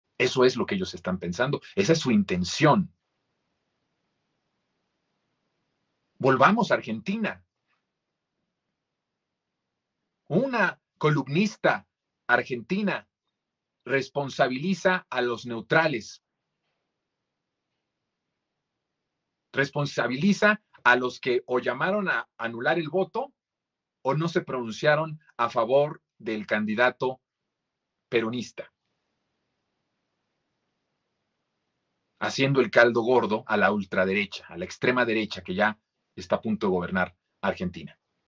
EL NEUTRALISMO LE HIZO EL CALDO GORDO A LA ULTRADERECHA DE ARGENTINA.
Jonathan López menciona que gracias a la neutralidad del pueblo argentino y quienes no se pronunciaron ante esta situación le hicieron el caldo gordo a la ultraderecha que está a punto de gobernar Argentina, es decir, Javier Milei ganó por el hecho de que muchos tibios no hicieron periodismo y no fueron oposición de acuerdo con una columnista del país.